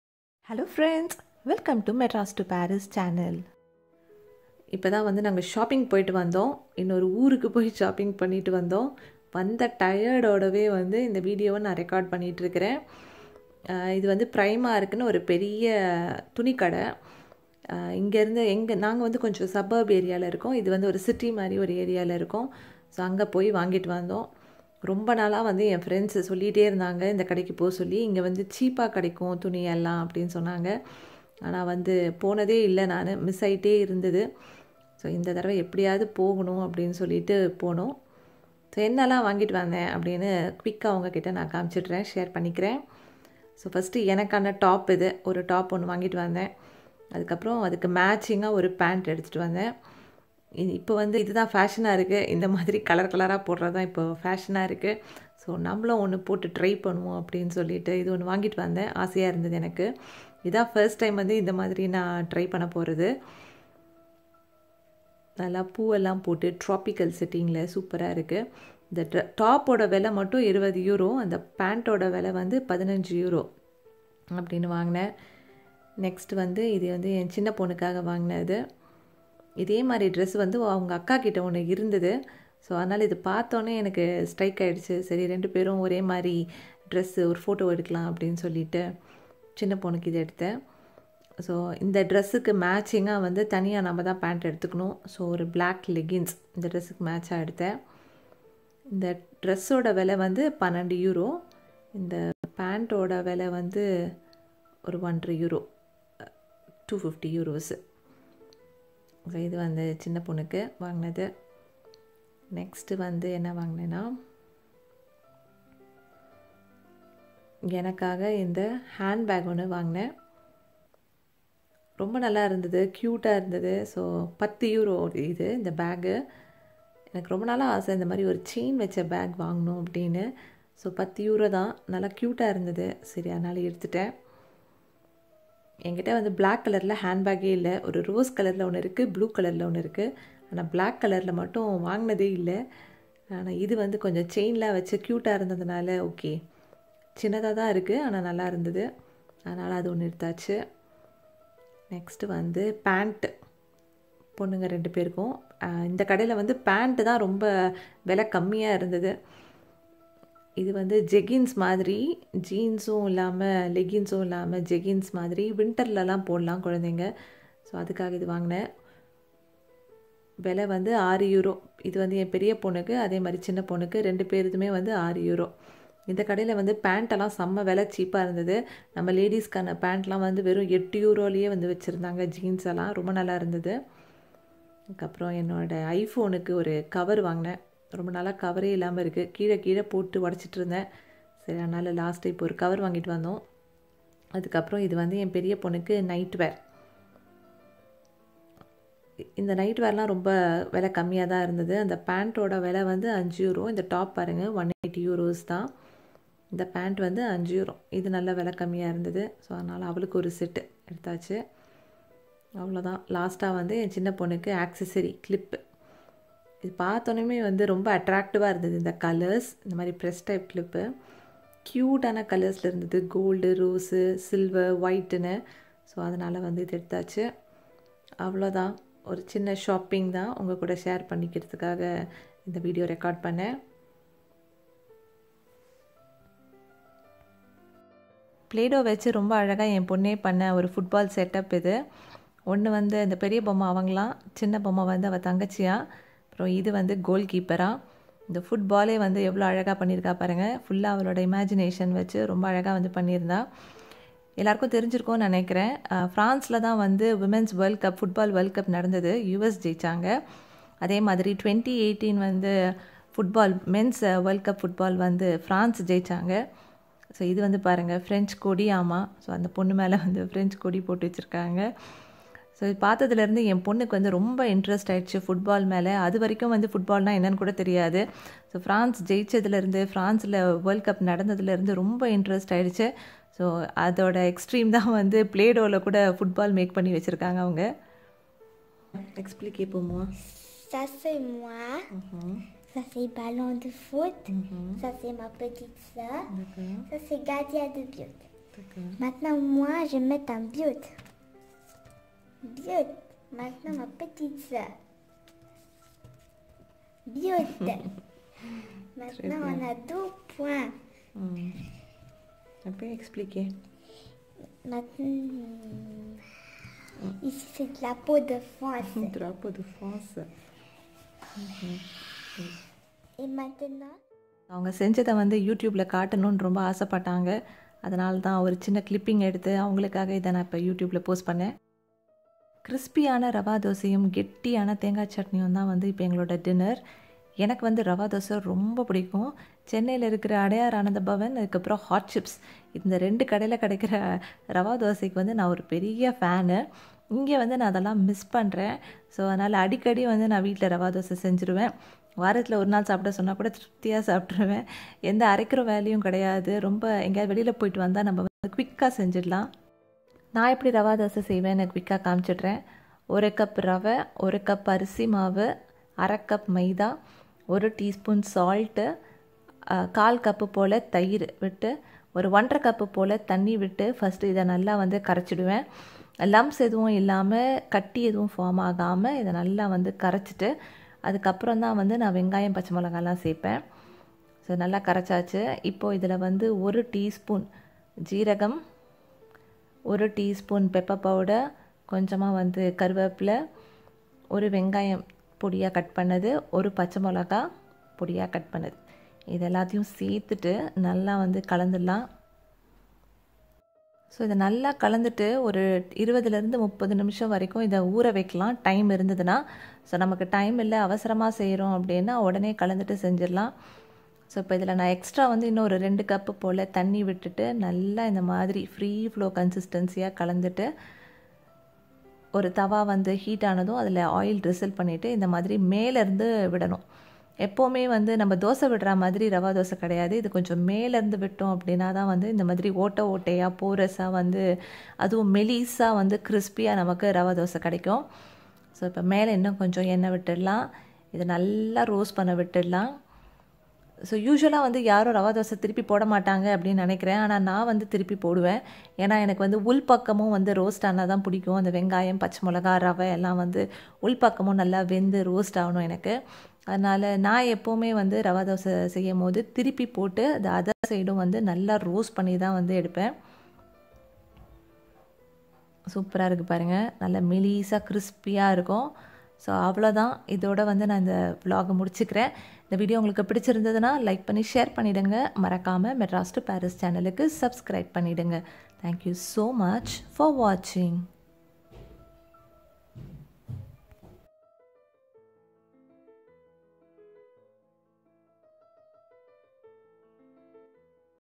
Tamil VLOG | Shopping Haul and Dinner routine vlog | France
I'm a Tamil Vlogger living in France. I post videos on Life in France,
Travel and Vacation Vlogs, Teach French in Tamil ,
Post Traditional and International recipes...etc